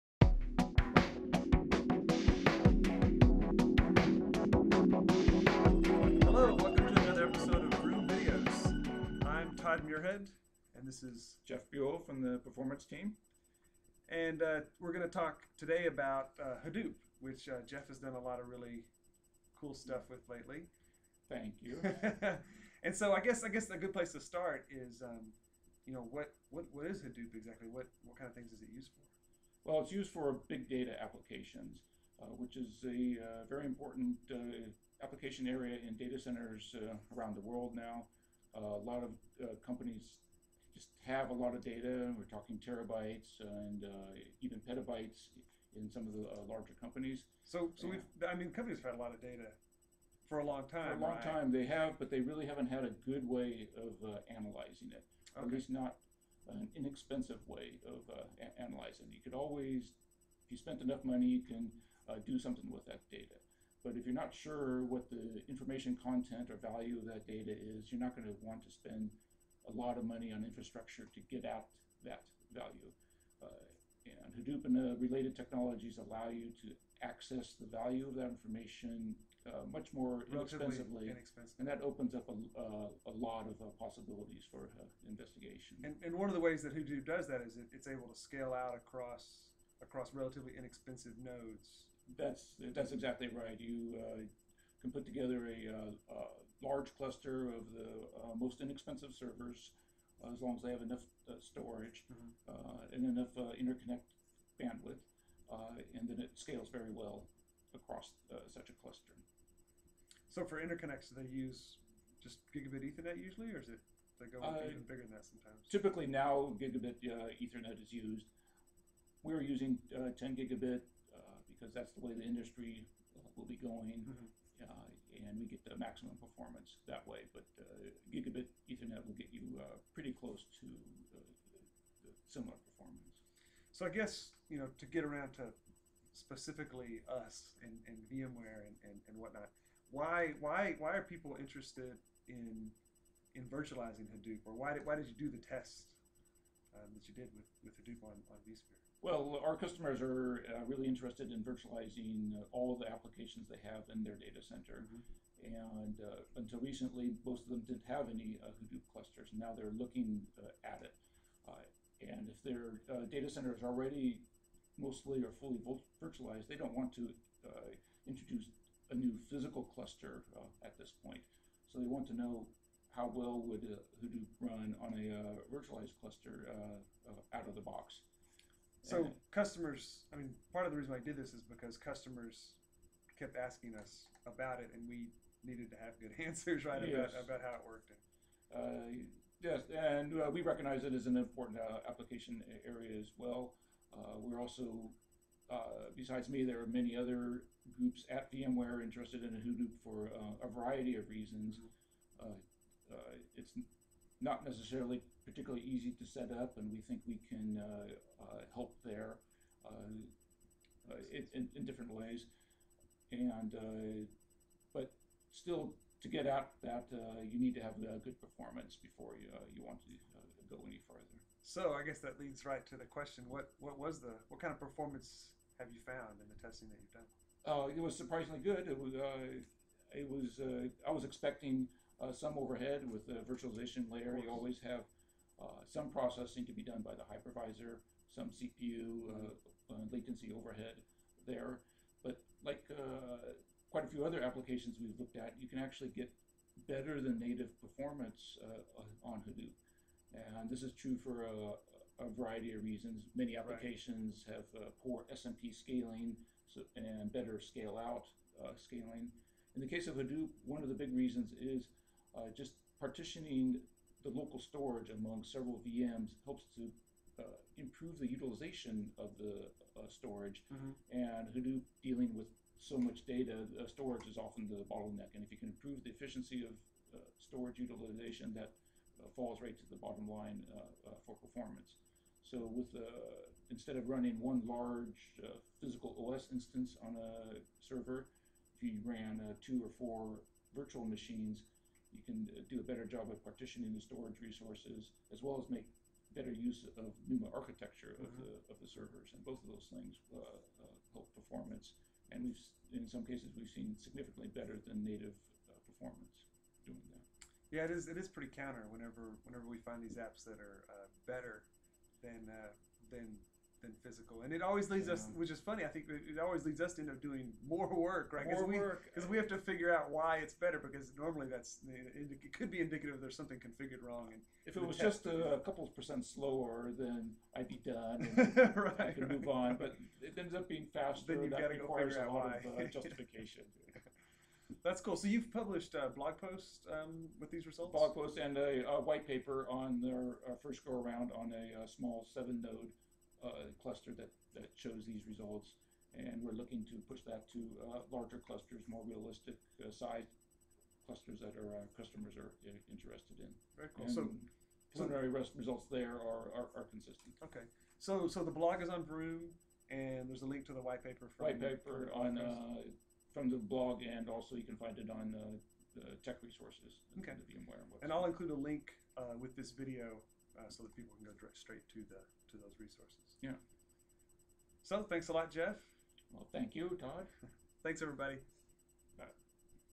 VMware VROOM! Video Episode 3: Hadoop
Discussion about performance of Hadoop on vSphere 5 with some talk about recent results.